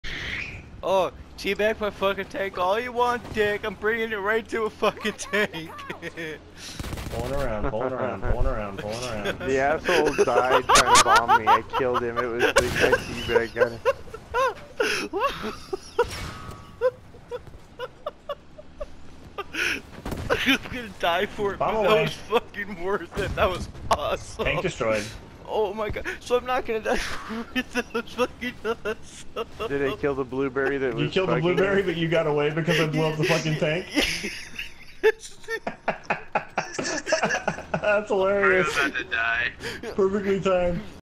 Roadkilling trolls
Some dude, in an effort to troll, sits on my tank. I prove to him why that's a bad idea.